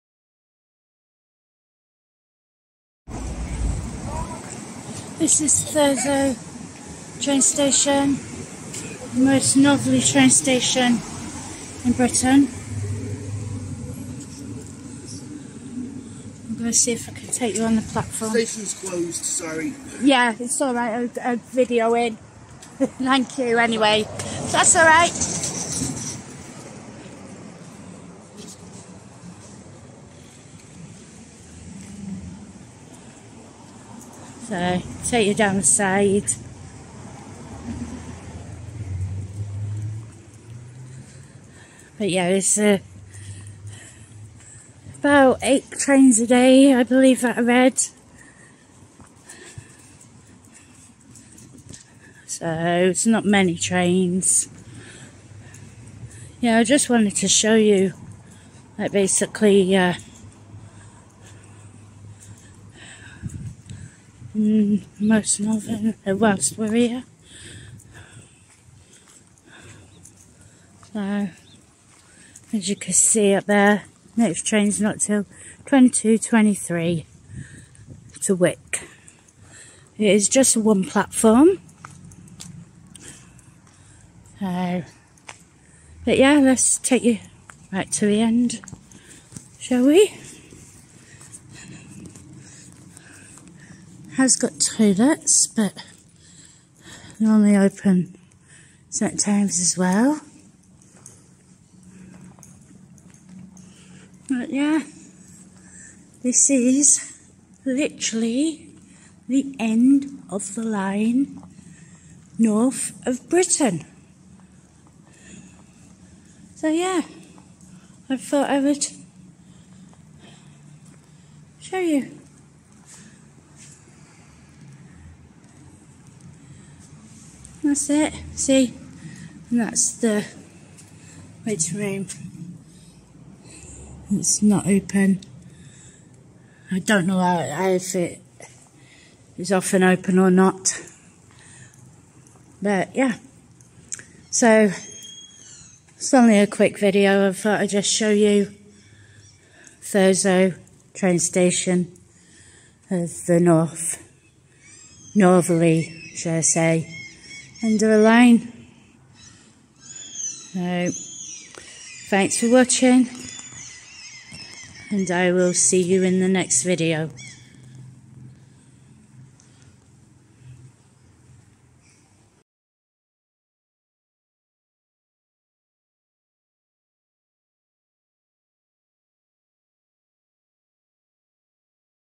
This is the end of the line
This is the end of the line in Great Britain the most northern train station in Great Britain.
Thurso is the last train station at the top of Scotland.